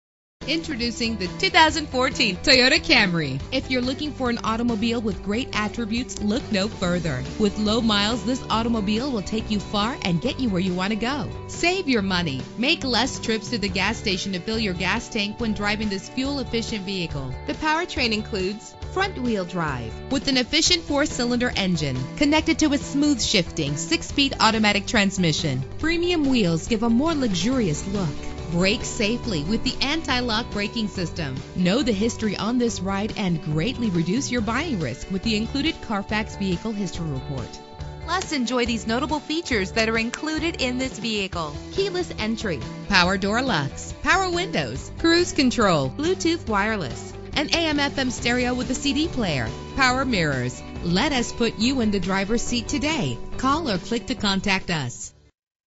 2014 Toyota Camry - Slidell LA
This vehicle represents similar vehicles available at Toyota of Slidell.
Year: 2014
Make: Toyota
Model: Camry
Trim: SE
Engine: 2.5 liter inline 4 cylinder 16 valve MPFI DOHC
Transmission: 6-Speed Automatic
Color: White
Mileage: 16482
Address:
300 Howze Beach Rd